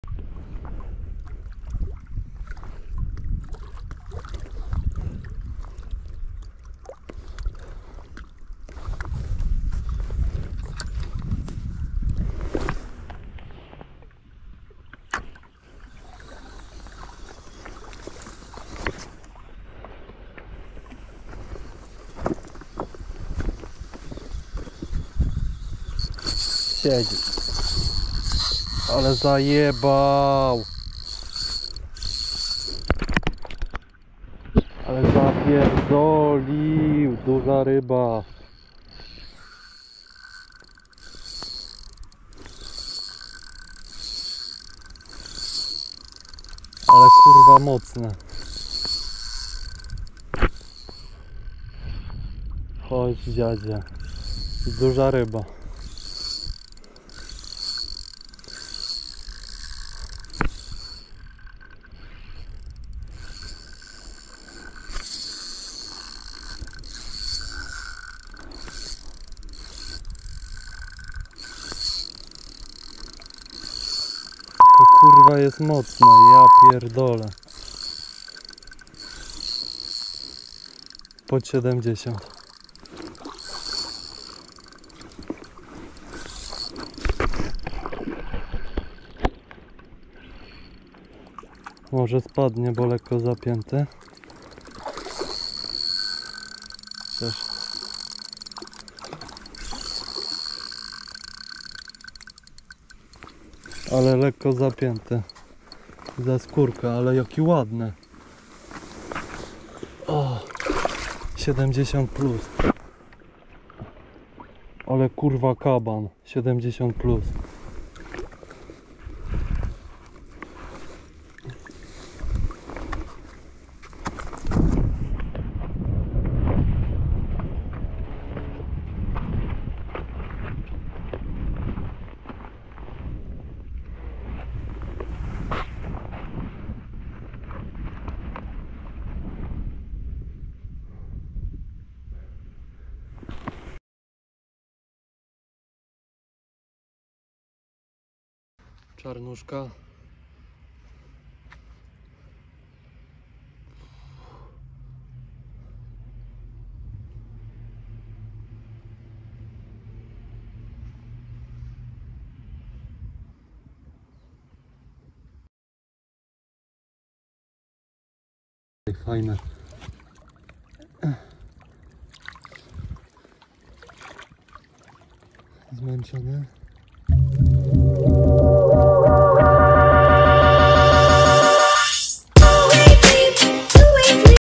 Piękny wiosenny boleń z jeziora 74cm i moc ataku na przynętę ,polskie GT! Aspius on topwater
Boleń z jeziora i cały hol + zwrócenie wolności…
Sprzętowo :
- suzuki rxf
-daiwa certate 4000
-plecionka momoi 1pe
-przynęta własnego wyrobu o nazwie : MACZETA 🗡️